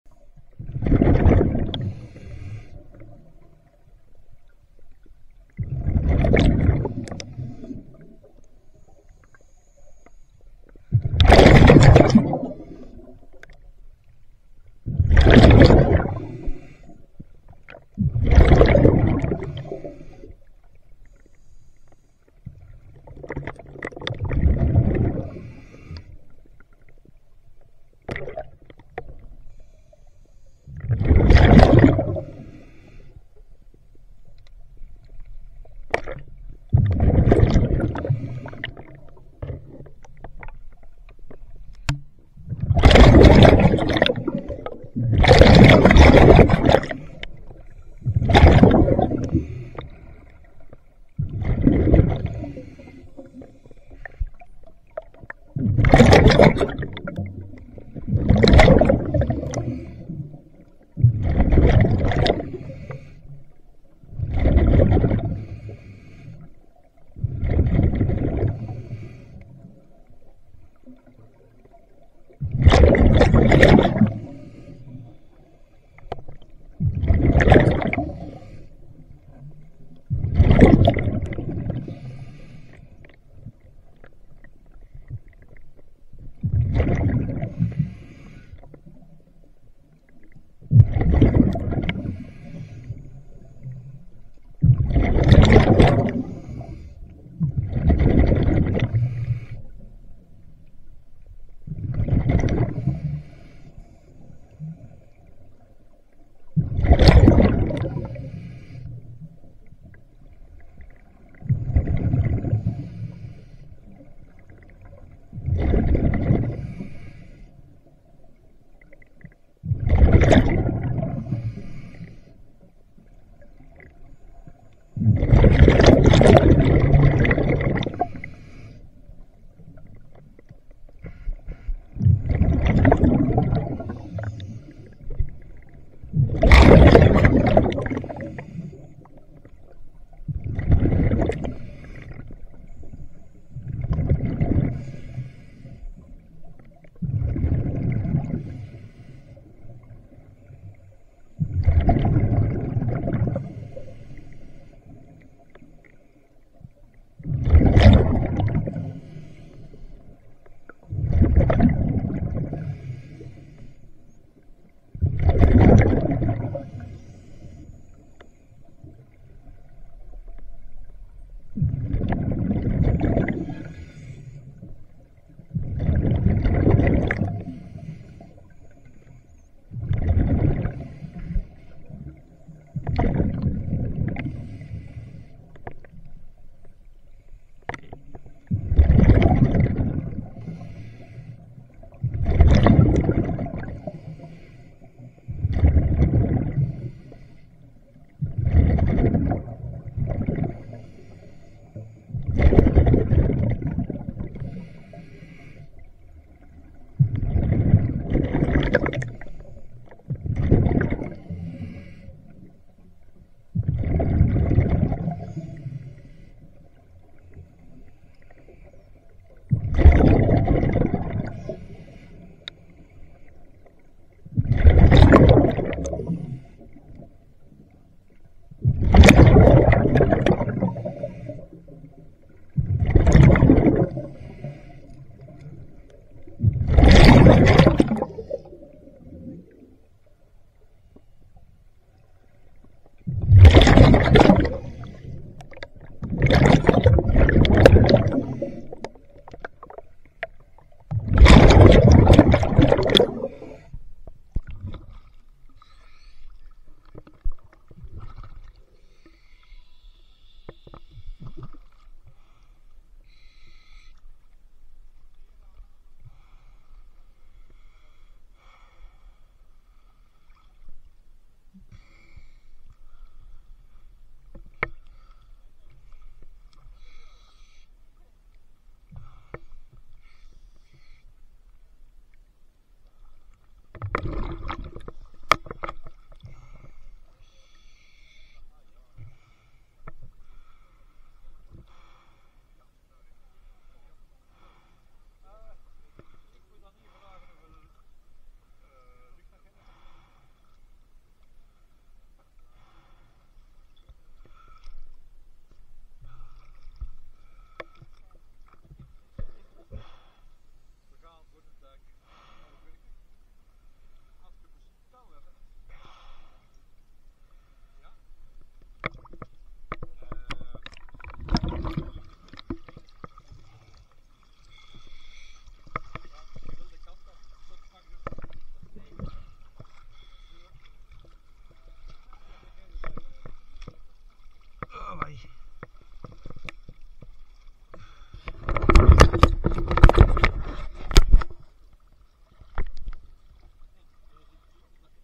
2024 06 08 put van Eekeren Met Marc headcam part 5
a relaxing dive with Marc from punt alfa vzw
we saw some nice carp, pike, loads of small fish